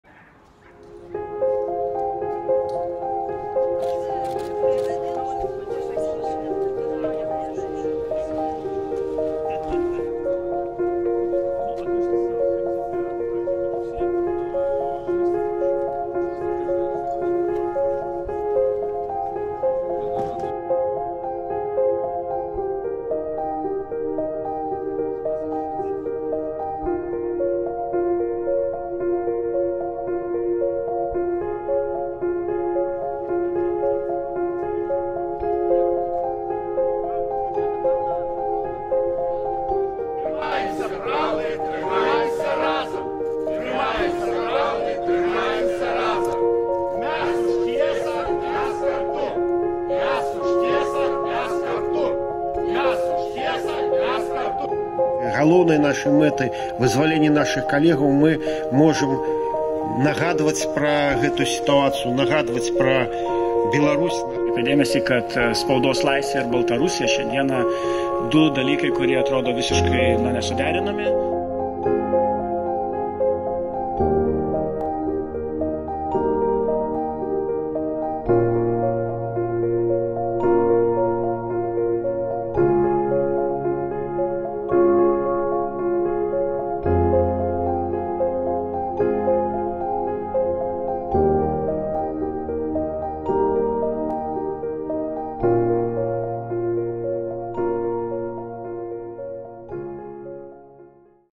Не даць забыць тых,хто за кратамі: акцыя салідарнасці ад беларускіх і літоўскіх журналістаў 3 траўня
3 траўня — у Міжнародны дзень свабоды прэсы — медыйшчыкі, праваабаронцы, актывісты з Беларусі і Літвы прыйшлі ў былую вязніцу, каб выказаць салідарнасць са зняволенымі беларускімі журналістамі.

Акцыю салідарнасці арганізавалі Беларуская асацыяцыя журналістаў і Саюз журналістаў Літвы.

Старшыня БАЖ Андрэй Бастунец падкрэсліў:  “Пакуль мы не можам дабіцца галоўнай мэты — вызвалення нашых калегаў. Але ў нашых сілах нагадваць пра нашу трагічную сітуацыю, нагадваць пра Беларусь, нагадваць пра беларускіх журналістаў за кратамі і наогул пра ўсіх беларускіх палітвязняў».

Сумесная акцыя БАЖ і СЖЛ завяршылася сімвалічным вызваленнем беларускіх журналістаў-вязняў з Лукішскай турмы.